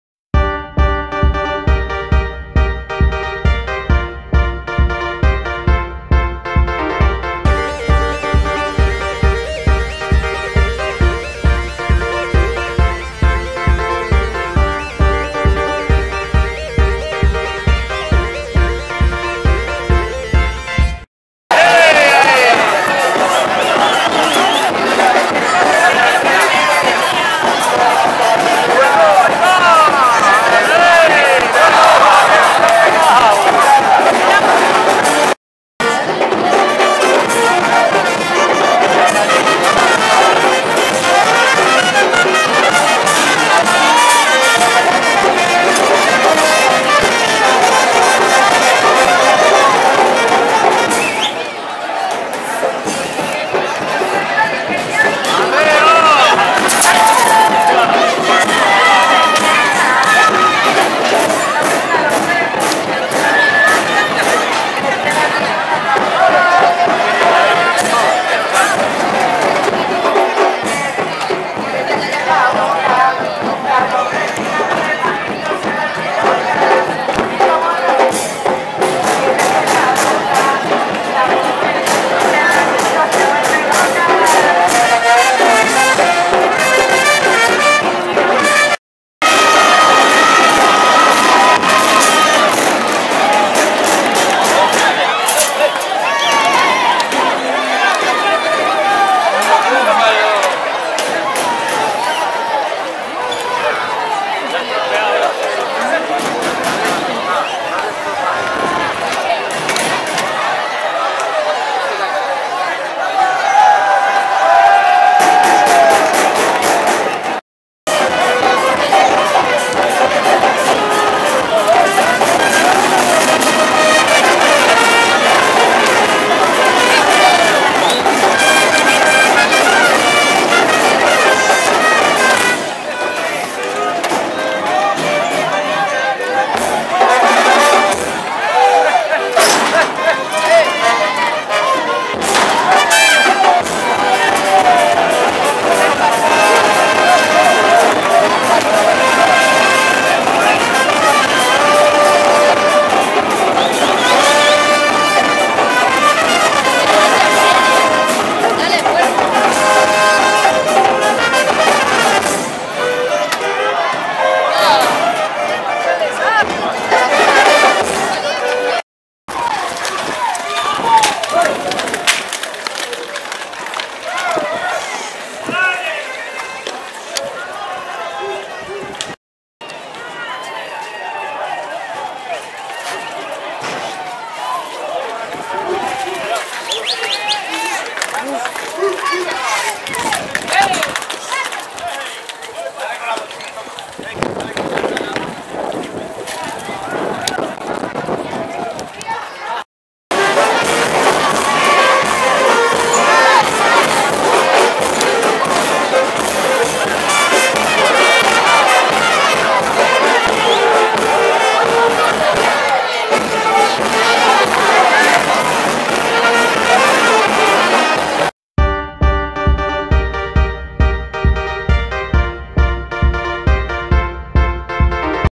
Encierro noche domingo 26-7-2015 Matapozuelos (Va)
Encierro nocturno, vistoso, con 3 toros. Continuas subidas y bajadas, buenas carreras. La única pega es que dos toros no querían entrar a chiqueros, ni con la ayuda de los mansos ni sin ella.
Han tenido que pasar varios intentos hasta que han sido conducidos a toriles.
Y lo mas importante, compartirlo con todo el mundoo. Un saludo!